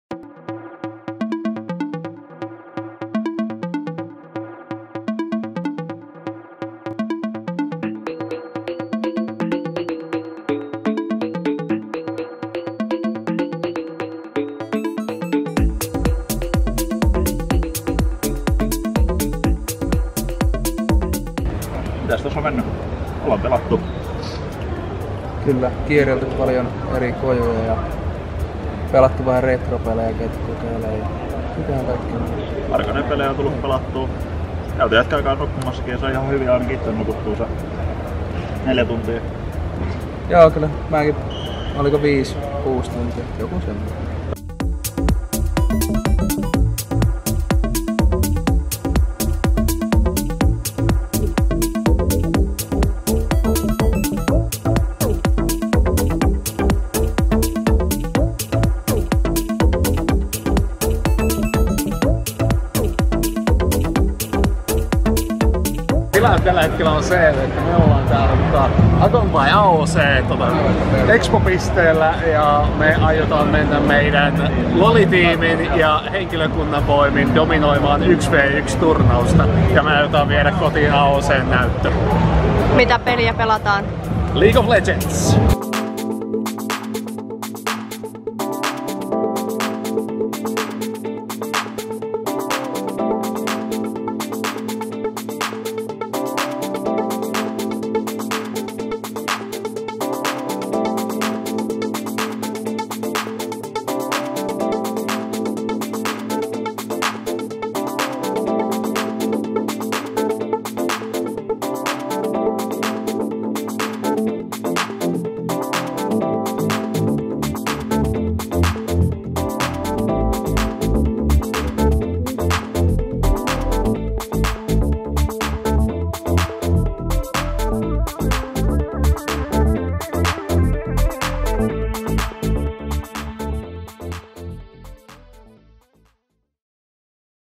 GamePit by Jamk @ Assembly Summer'22
GamePit 💜 Assembly!

Miltä näyttää, kun Jyväskylän ammattikorkeakoulun GamePit Esports Crew sekä joukkueellinen opiskelijagamereita viettää kesäistä loppuviikkoa Assembly Summer 2022 -tapahtumassa?

Tsekkaa video, niin tiedät!

Lue lisää GamePitistä ja vuokraa e-urheilun huipputilat käyttöösi osoitteessa jamk.fi/gamepitpro